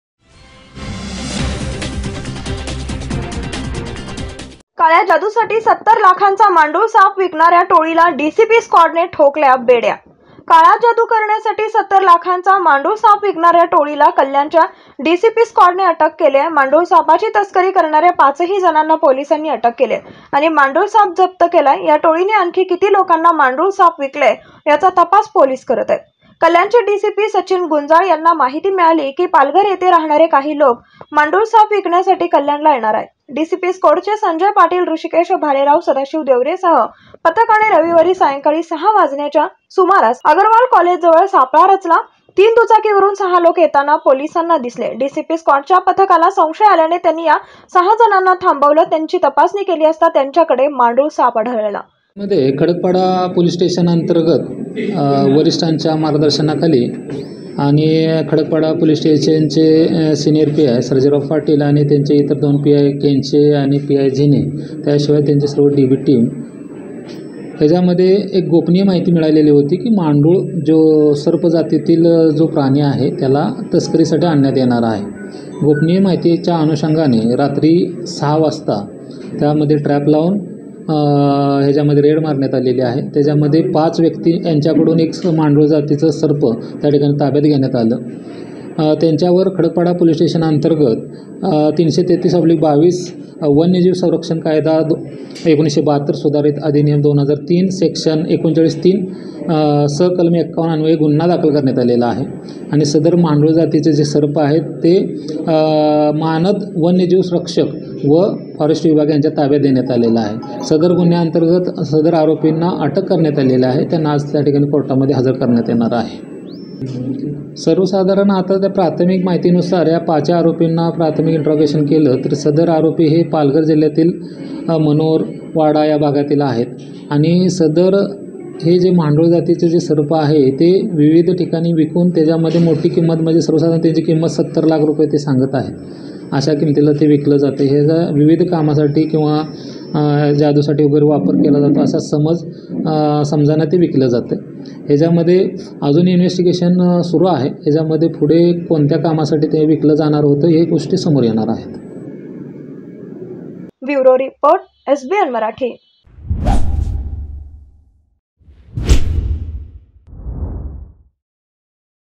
KALYAN | काळ्याजादूसाठी ७० लाखांचा मांडूळ साप विकणाऱ्या टोळीला कल्याण डीसिपी स्कॉडने ठोकल्या बेड्या
काळ्याजादूसाठी ७० लाखांचा मांडूळ साप विकणाऱ्या टोळीला कल्याण डीसिपी स्कॉडने ठोकल्या बेड्या